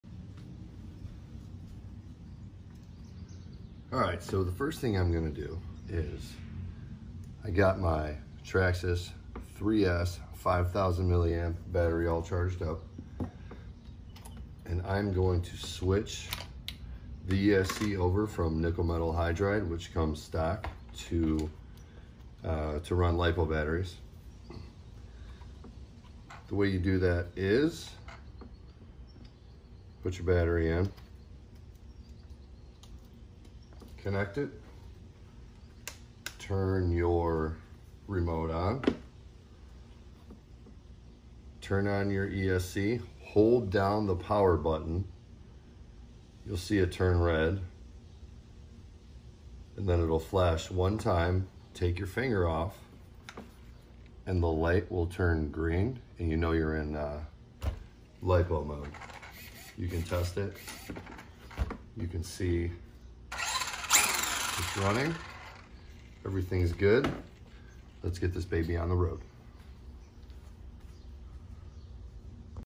Traxxas Slash VXL
How to change your RC car from nimh to Lipo mode. Traxxas Slash Lipo mode.